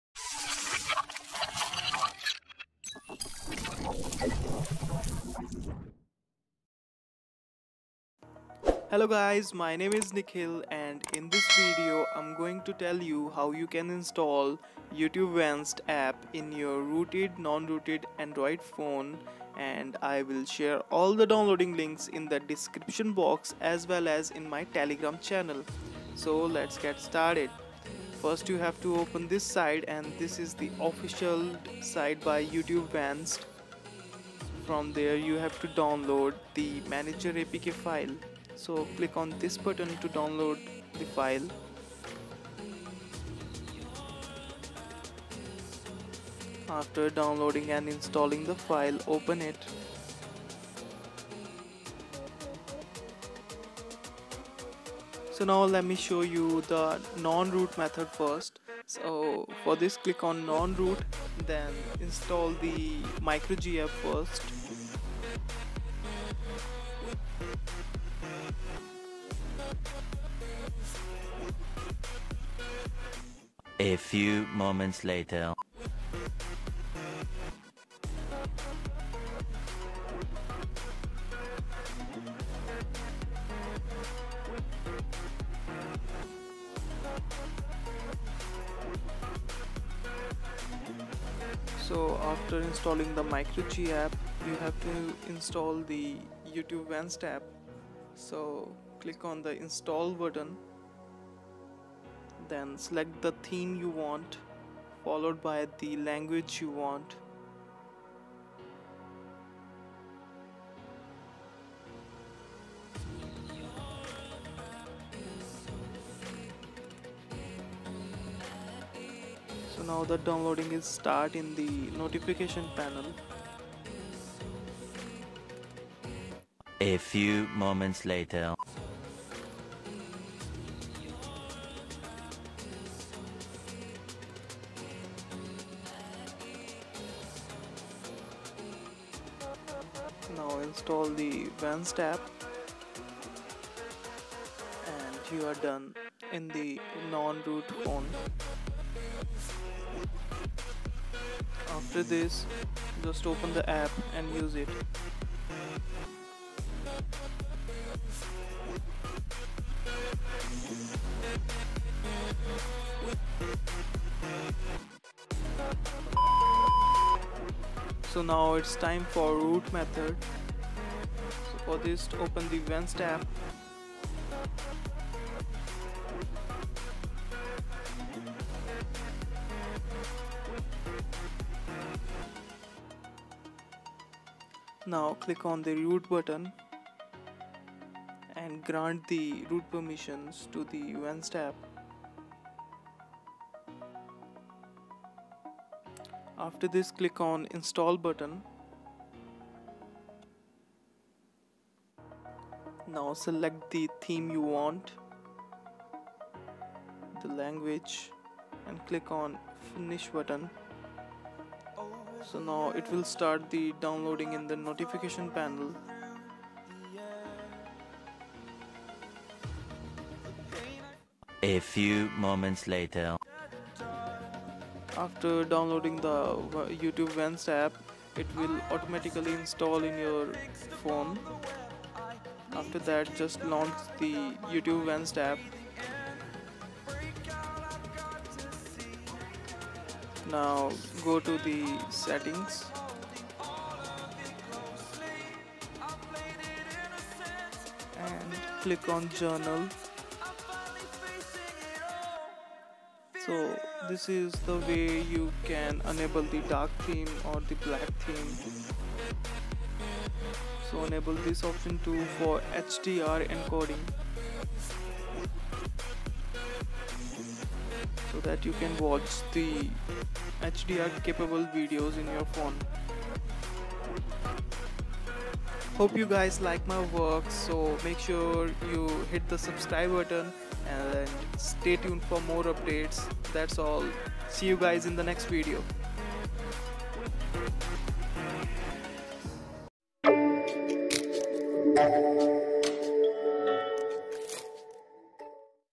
How To Install YouTube Vanced In Any Android Phone | YouTube Mod For Both Root / Non-Root (2020) 🔥
How To Install YouTube Vanced App In Any Android Phone | YouTube Mod App For Both Root / Non-Root (2020) 🔥🔥🔥

Non-Root Installation Method :
*Click on non-root.

1). Install The MicroG App.
2). Install The YouTube Vanced.
3). Thats All 🔥.

Root Installation Method :
*Click on root then grant the root permission to vanced manager app.

1). Install The YouTube Vanced
2). Thats All 🔥.

Enjoy The Ad. Less stream

**Note for Redmi, Xiaomi Phones
 Go to vanced manager app settings ~ battery optimization ~ no restriction ***

How To Enable Extreme FPS With 16xMSAA In PUBGM| FPS Meter | PUBG Mobile 2020 | No BAN 🔥 :-